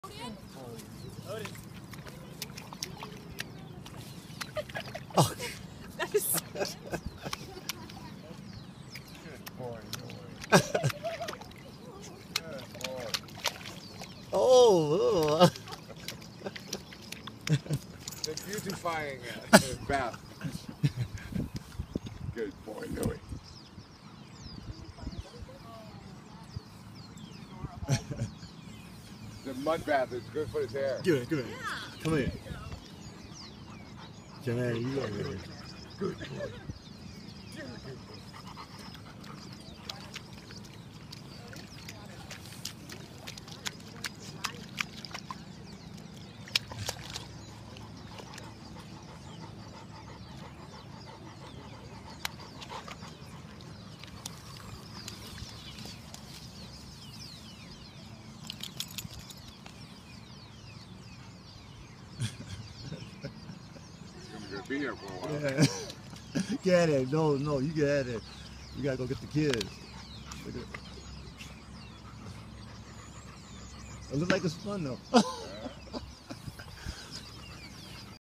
Labrador Puppy playing in mud
Labrador puppy playing at the dog park was pretty funny to watch how fun this dog was having.